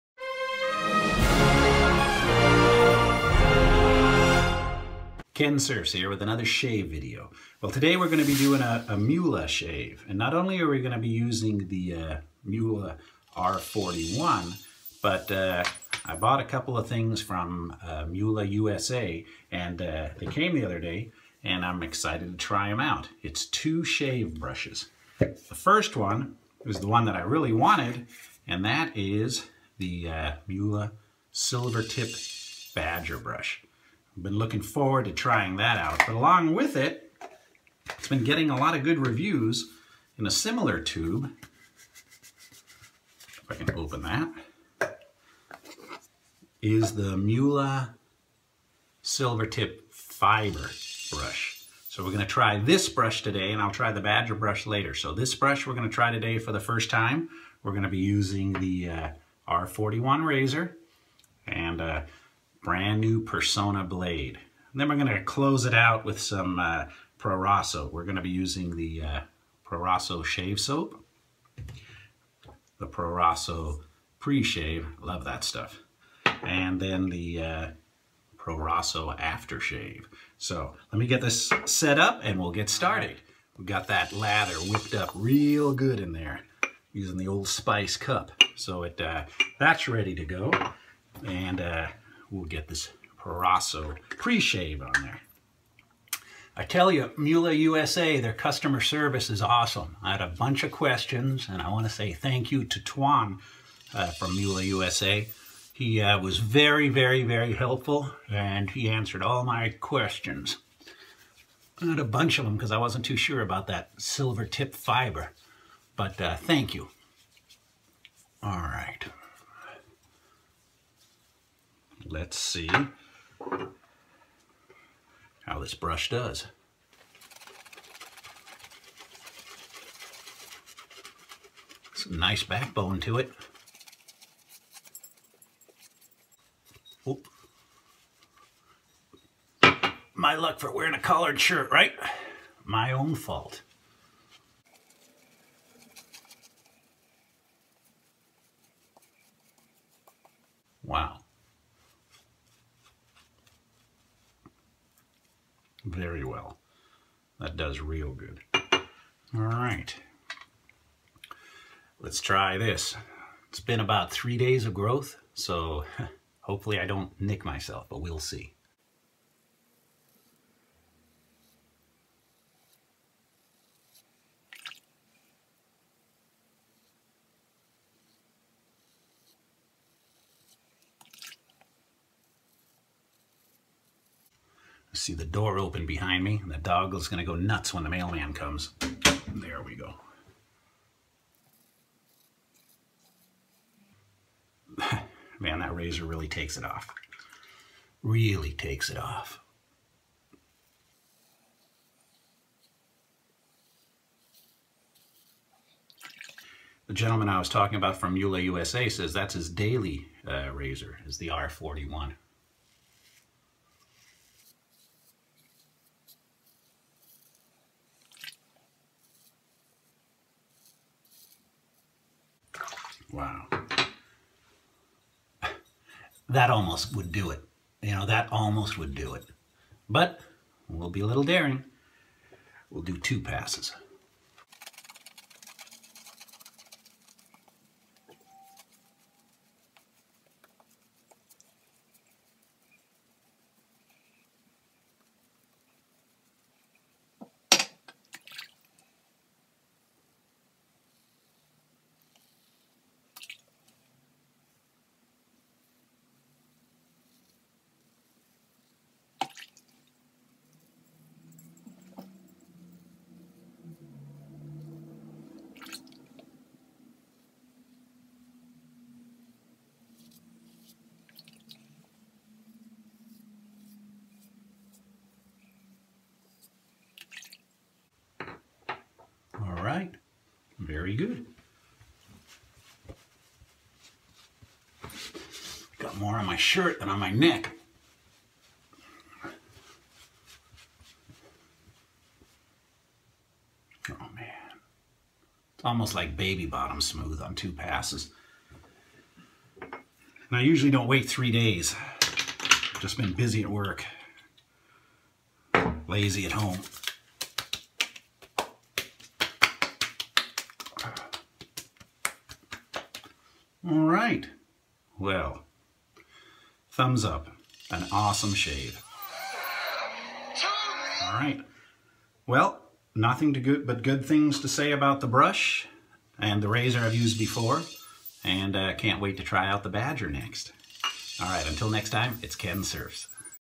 Muhle Silvertip Fibre Brush and R41 Safety Razor. Trying out the brush for the first time.
First time using the Muhle Silvertip Fibre shave brush along with the Muhle R41 Razor (2013 three piece version). Going to tackle 3 day growth. I've been lazy, so I hope that I don't cut myself.

Razor - Muhle R41

Blade - Personna

Brush - Muhle Silvertip Fibre

Preshave - Proraso Menthol

Shave Creme - Proraso Sapone Da Barba

Aftershave - Proraso Lozione Dopobarba